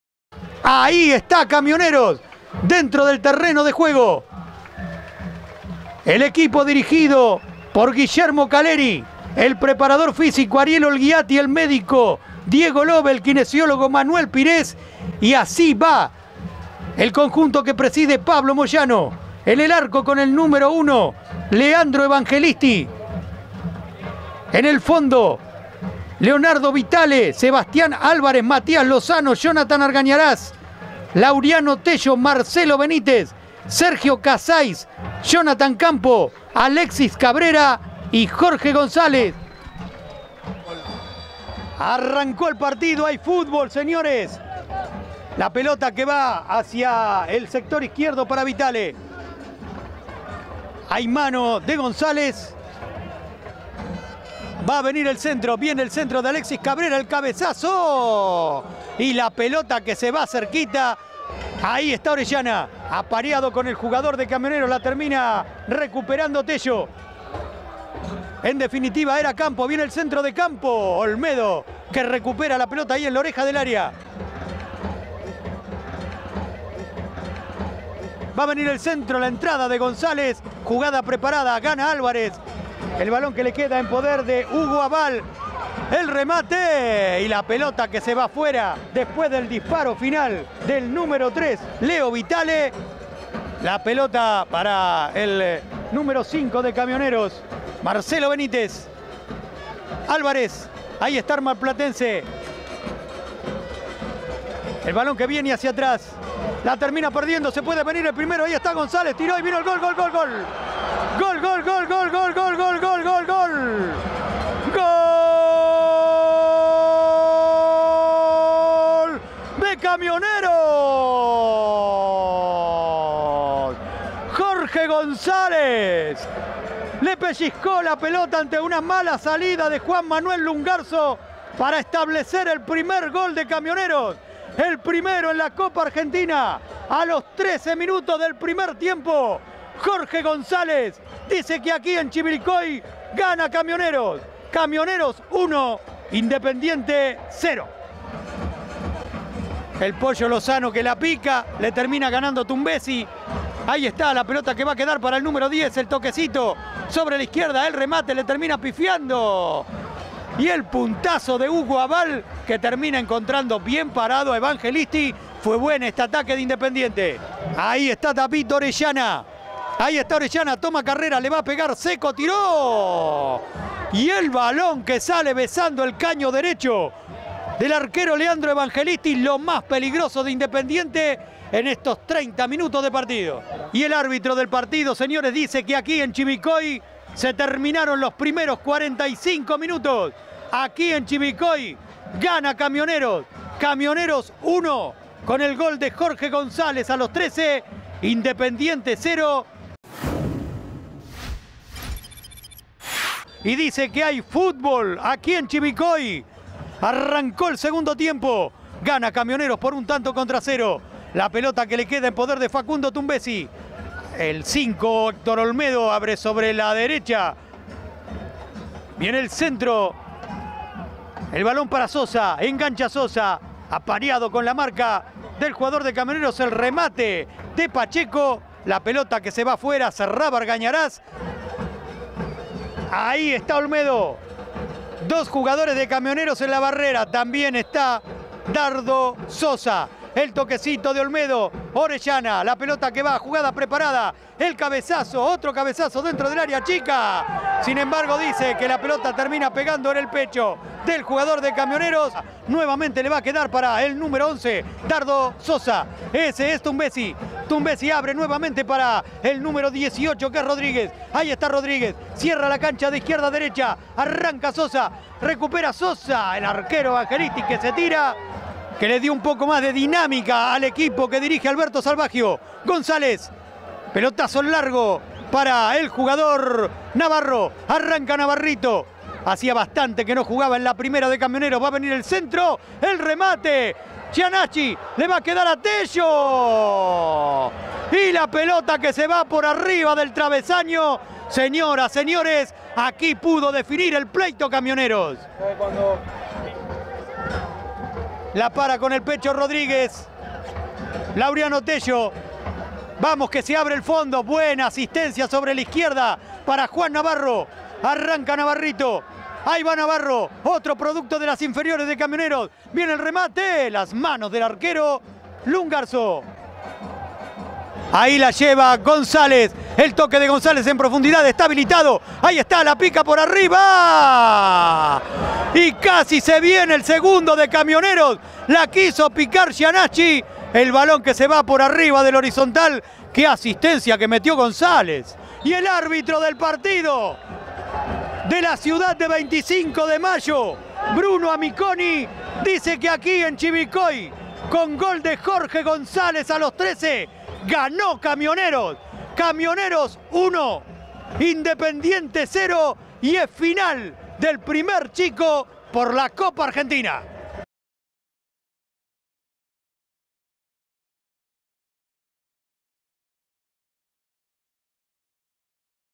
Copa Argentina Ida - Independiente de Chivilcoy 0 Club Camioneros 1
¡Nuestras redes!